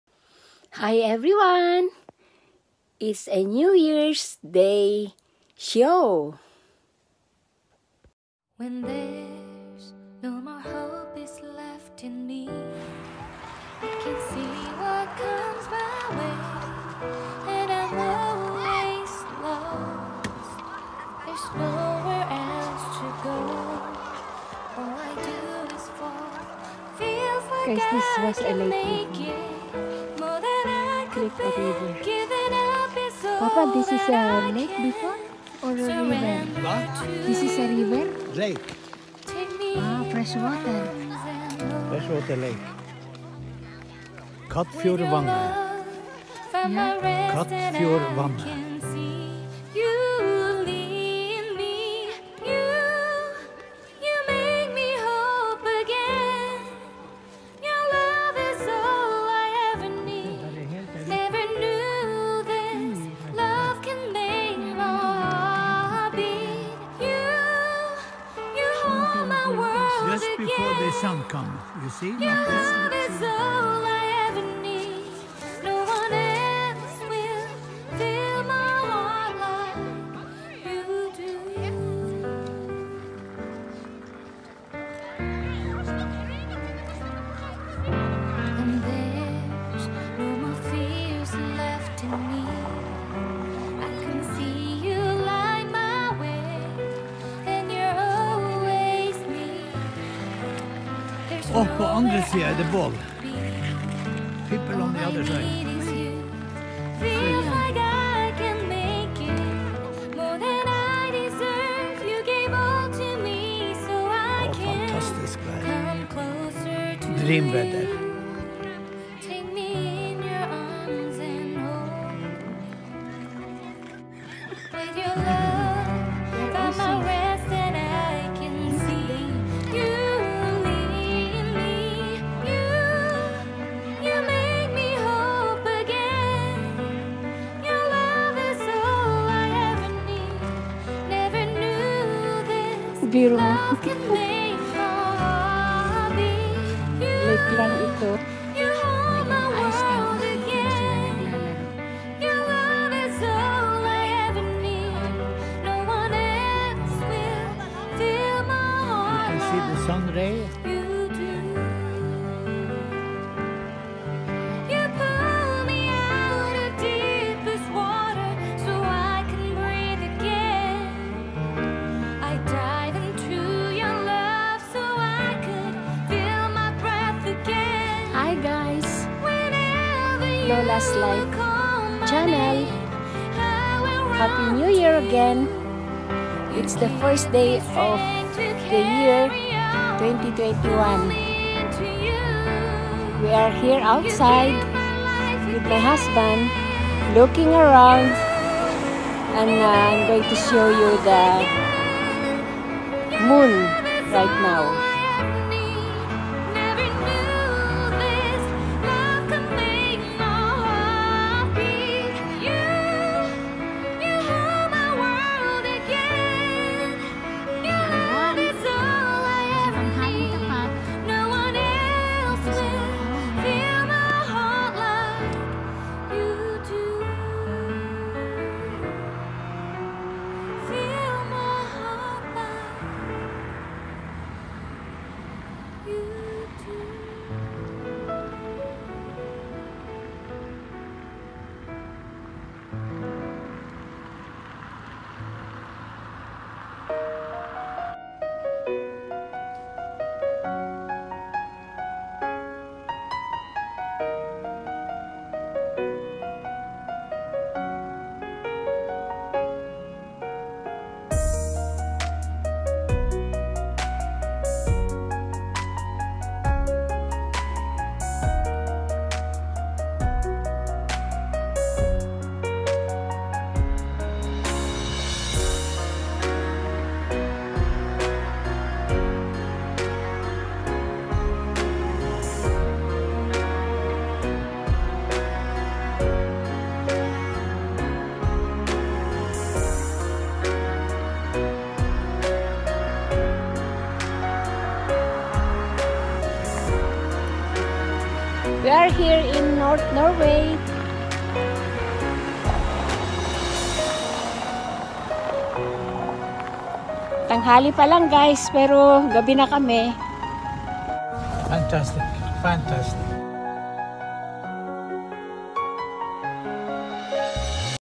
FROZEN LAKE for skating | KATFJORD LAKE | KATTFJORD VATNET | TROMSØ NORWAY | NEW YEAR’s Day 2021
Today is a New Year’s day, the first day to year 2021. Because of the pandemic, it is better to go outdoors and luckily we found the place full of family gathering, by means of skating to the frozen lake. It’s amazing show and how are resourceful people Are to spend quality time with each one despite of the cold weather. So happy guys, seeing people in the midst of the wilderness...